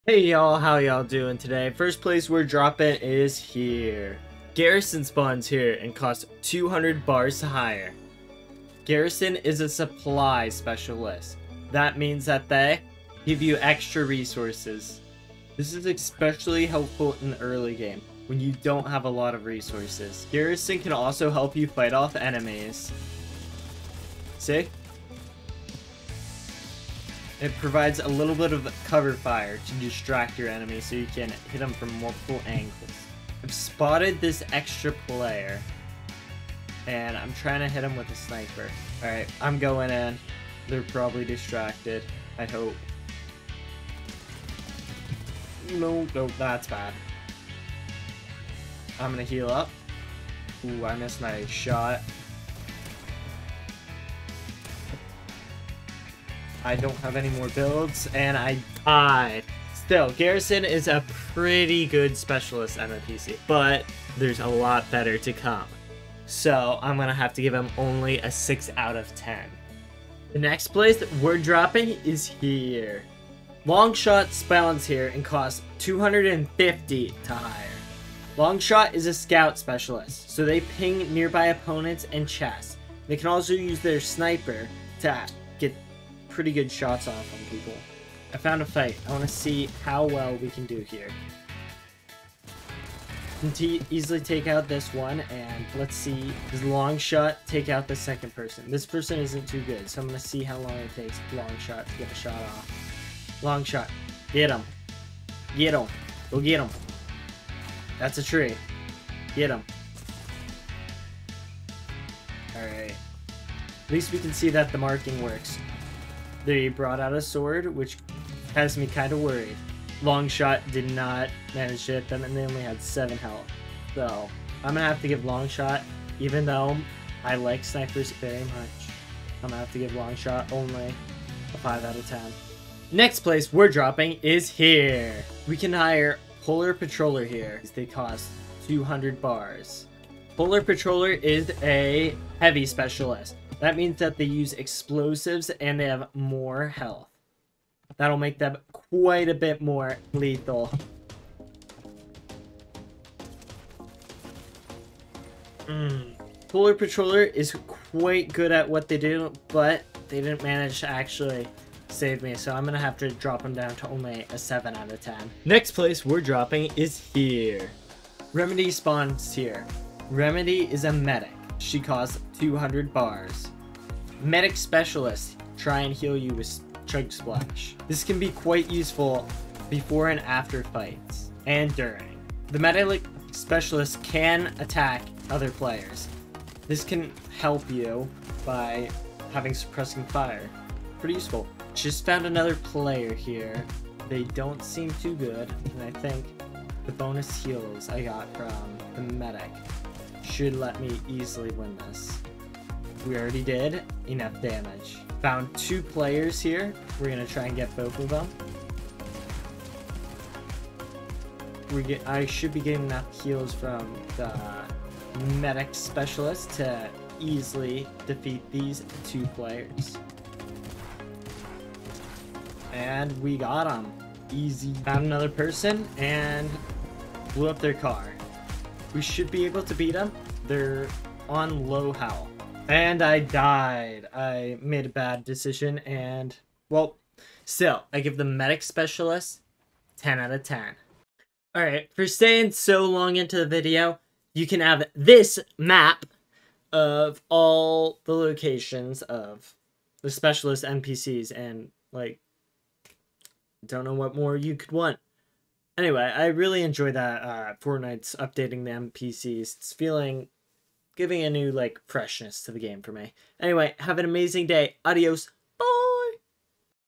NEW Specialist NPCs
Today we take a look at the new Specialist types of NPCs.
I used DaVinci Resolve to edit this.